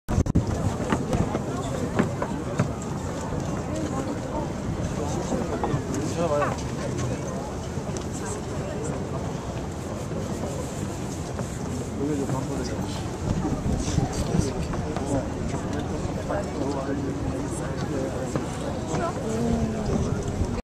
Glittering Seoul!!
Beautiful view from the tower deck at Namsan Tower. This is also where the Love Locks are located.